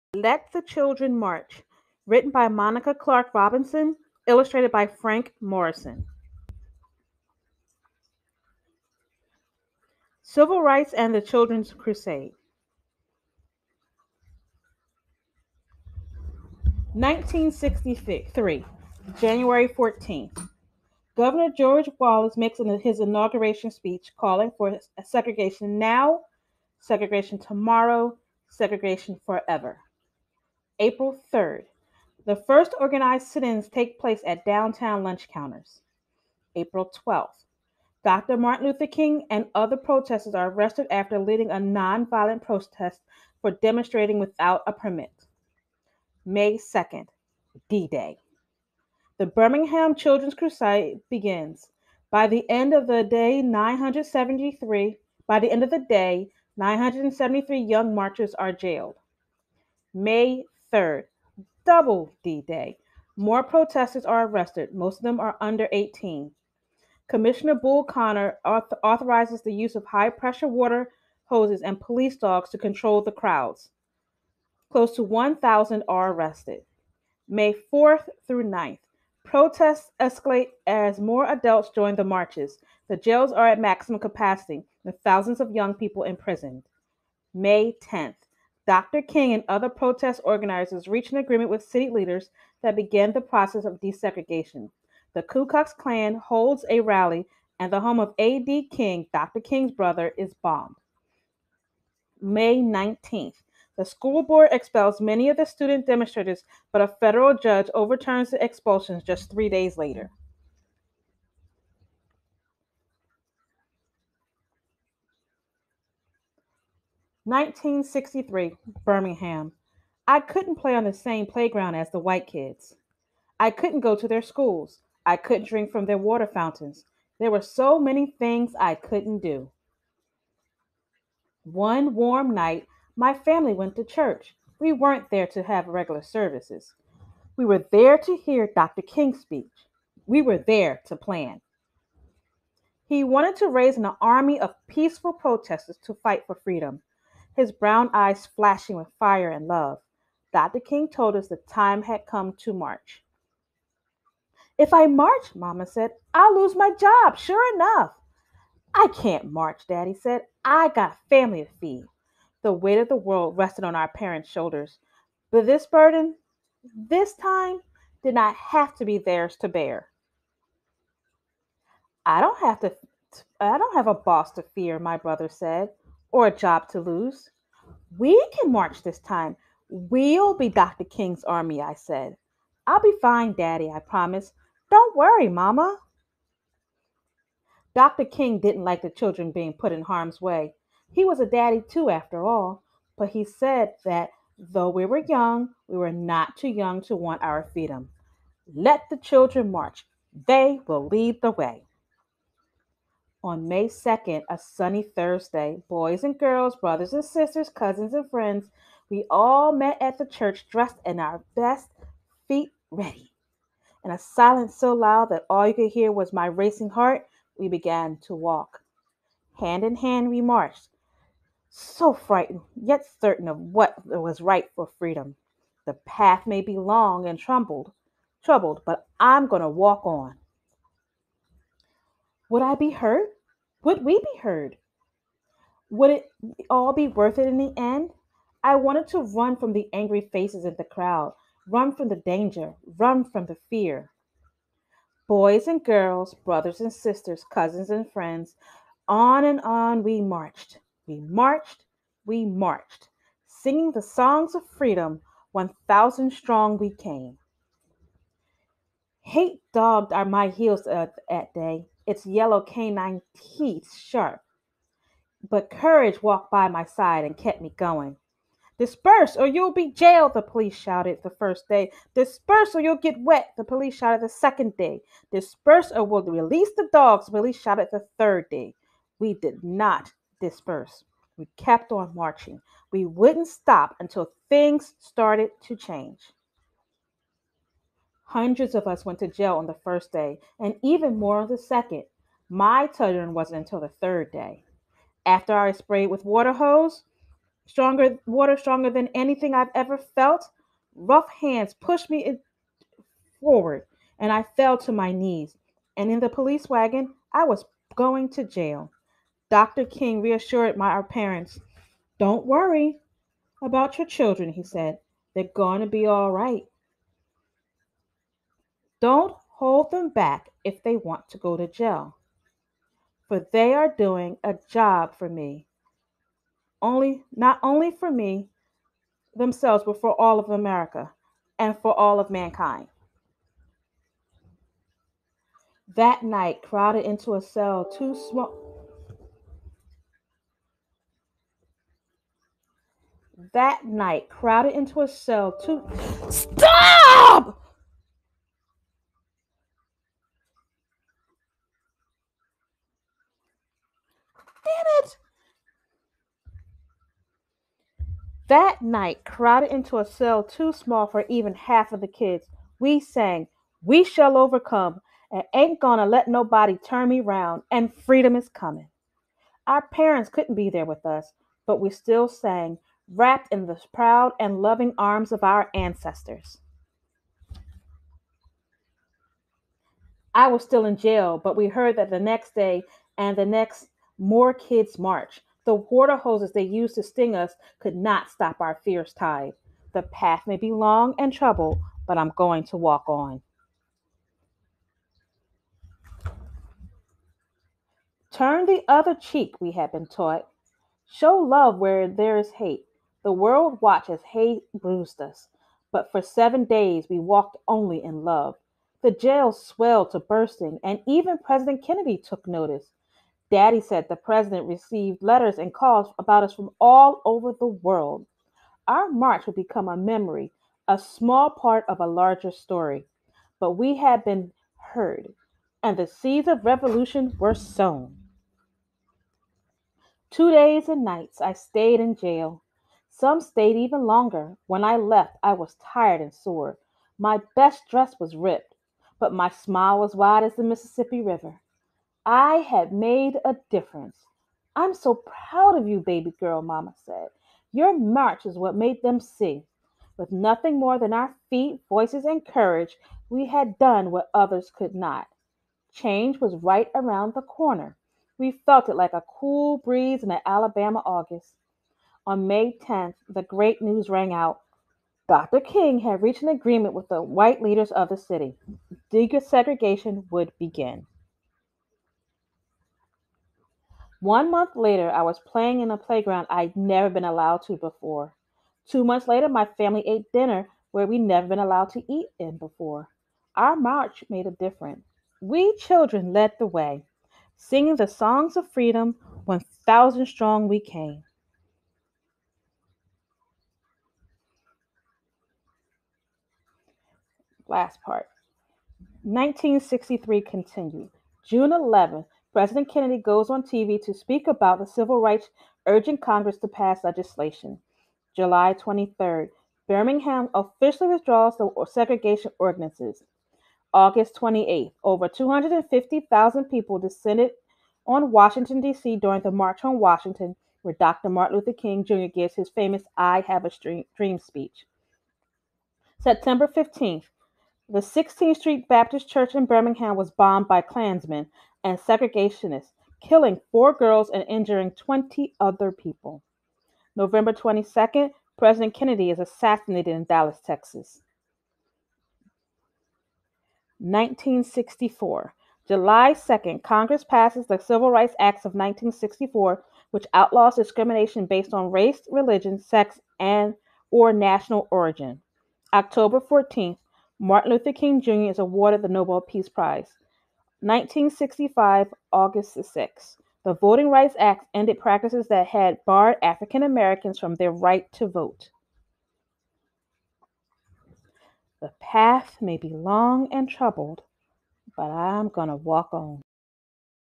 Let the Children March
In 1963 Birmingham, Alabama, thousands of African American children volunteered to march for their civil rights after hearing Dr. Martin Luther King Jr. speak. They protested the laws that kept black people separate from white people. Facing fear, hate, and danger, these children used their voices to change the world.